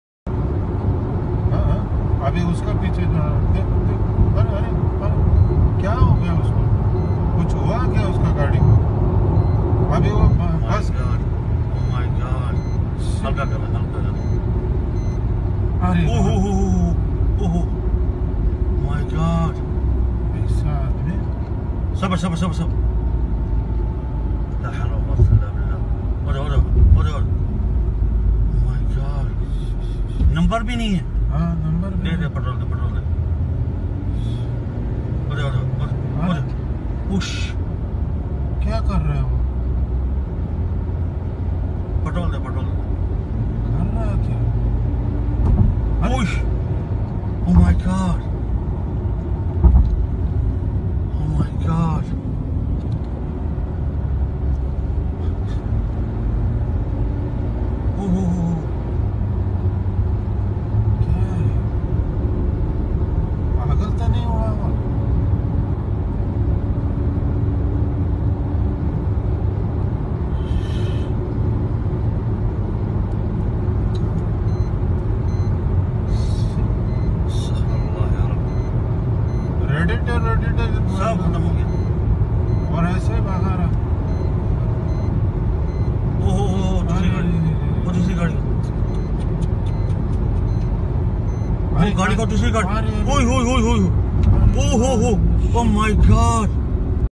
قائد مركبة يصطدم بمركبة من الخلف بشكل متعمد ويتسبب بانقلاب مركبة
قائد مركبة يصطدم بمركبة من الخلف بشكل متعمد  ويتسبب بانقلاب مركبة على أحد الطرق السريعة